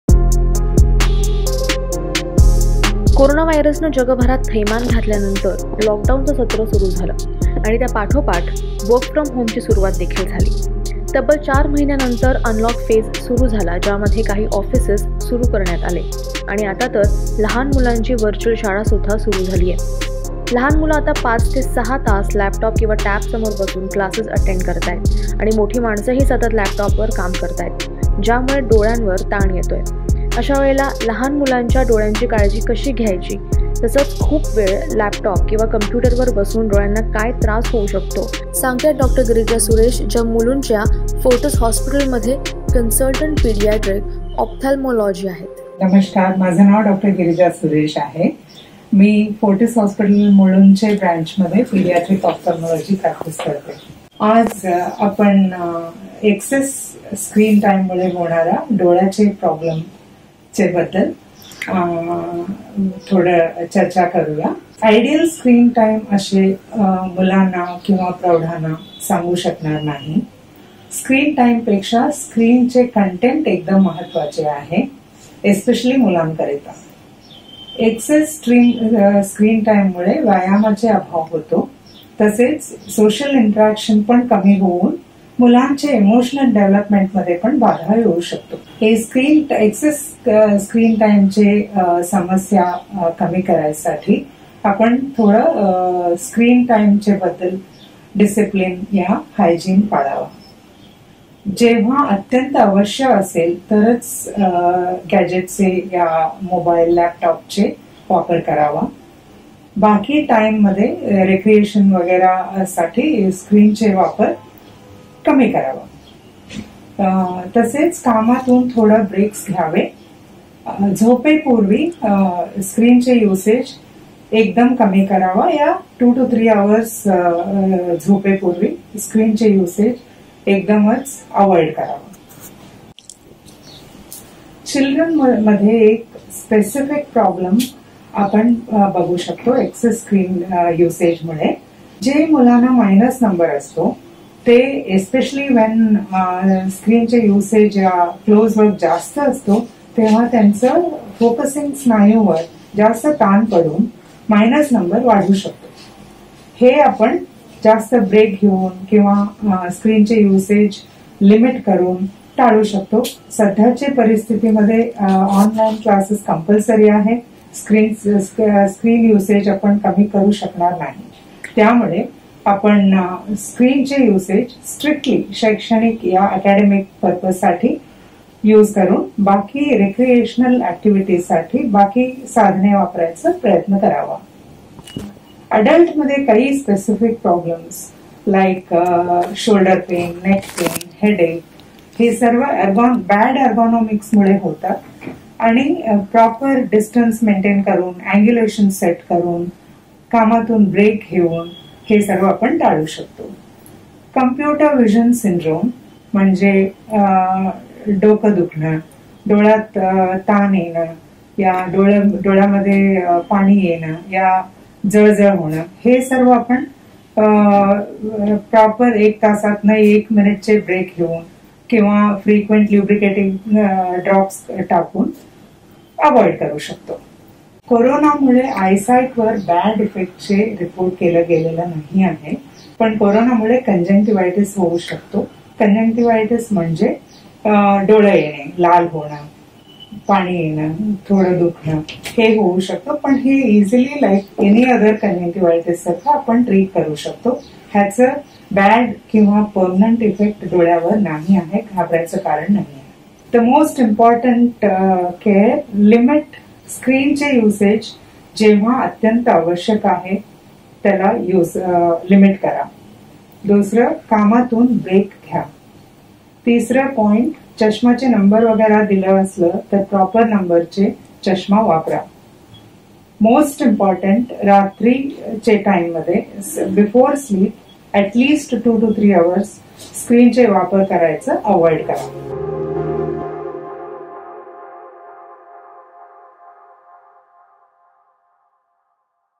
Digital Education Dangerous for Children I सावधान ! डिजिटल अभ्यासामुळे डोळ्यांवर 'हा' होतोय परिणाम
तब्बल ४ महिन्यानंतर, अनलॉक फेज सुरु झाला, ज्यामध्ये काही ऑफीसेस सुरू करण्यात आले आणि आता तर, लहाण मुलांची वरच्युल शाळा सुद्धा सुरू झालीये. लहाण मुलं आता ५-६ तास लॅपटॉप किंवा टॅब समोर बसून क्लासेस अटेंड करतायत मोठी माणसं ही सतत लॅपटॉपवर काम करतायत, ज्यामुळे डोळ्यांवर ताण येतोय